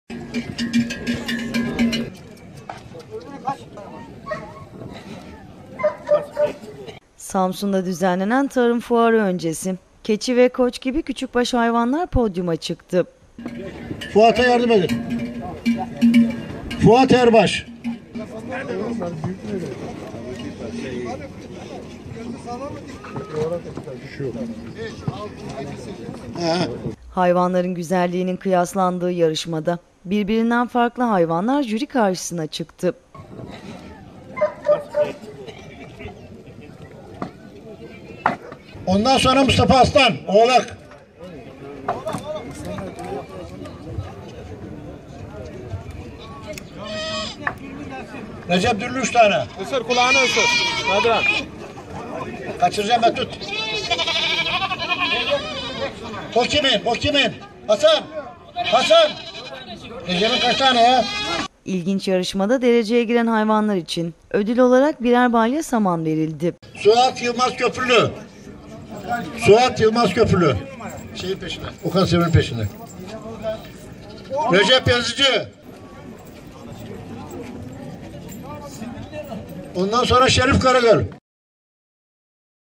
Samsun'da oğlak, teke, koç ve kuzular için güzellik yarışması düzenlendi
Samsun'da düzenlenen TÜYAP 3'üncü Tarım Fuarında, Samsun Damızlık Koyun ve Keçi Yetiştiricileri Birliği tarafından oğlak, teke, koç ve kuzular için güzellik yarışması düzenlendi.